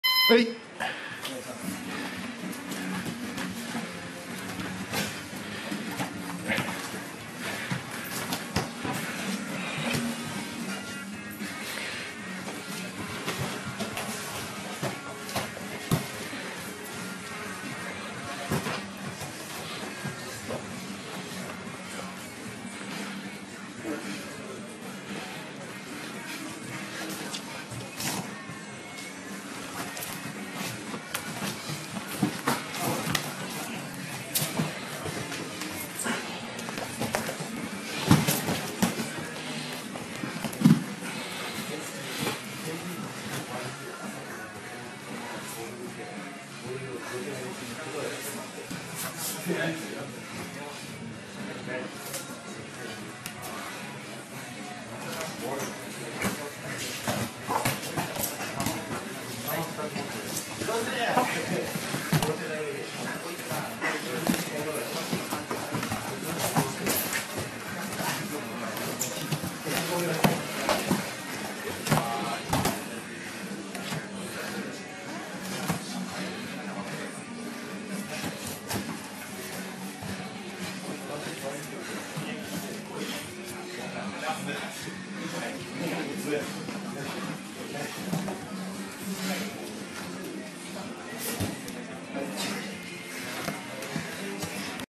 カミムラさんvsタカオカさん(2008-1-11)
2008年1月11日金曜日。駿河道場のスパーリングにて。カミムラさん62kgとタカオカさん78kgの対戦。